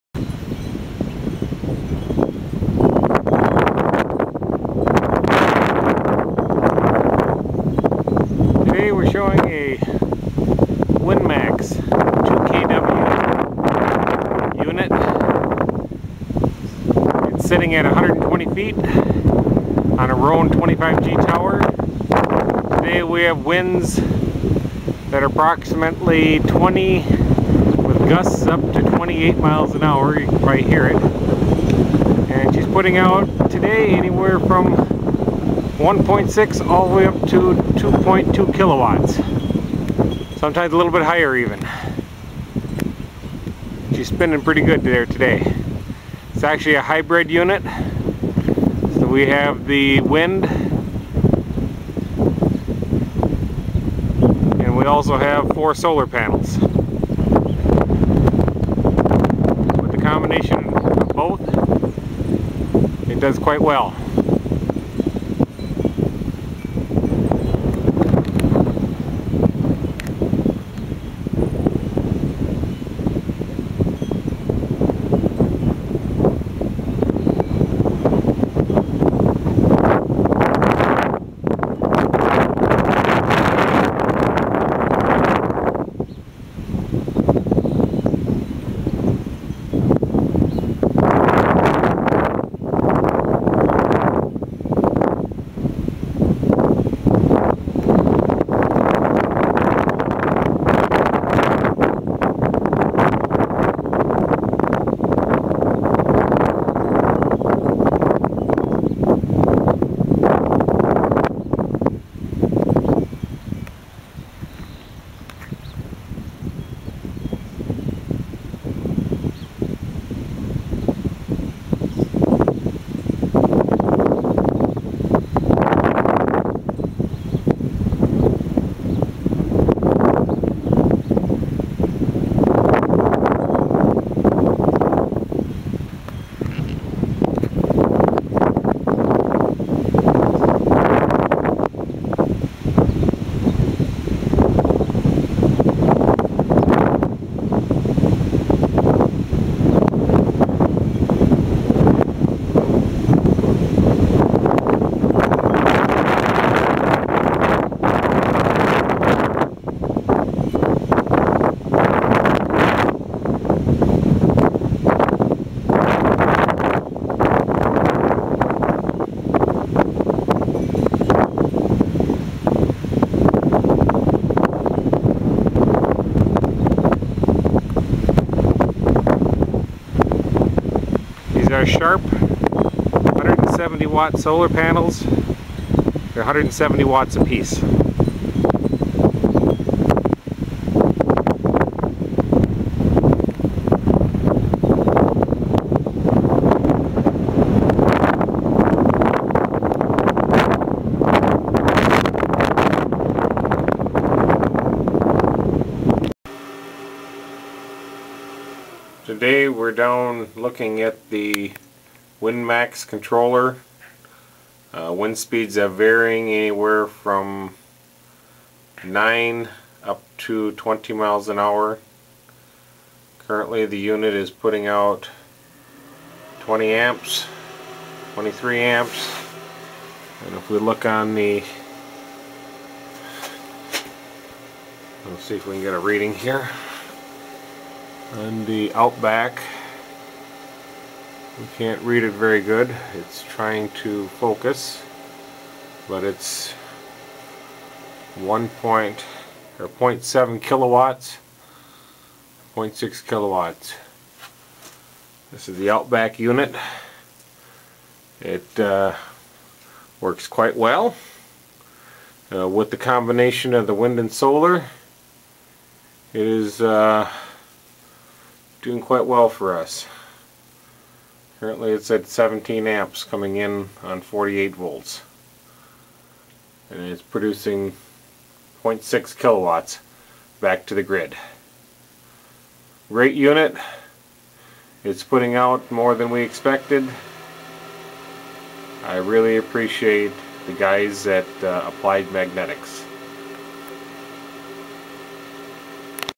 WindMax 2KW Grid Tie Wind Turbine Performance - day 2
Day 2 of the 2kW performance at different wind speed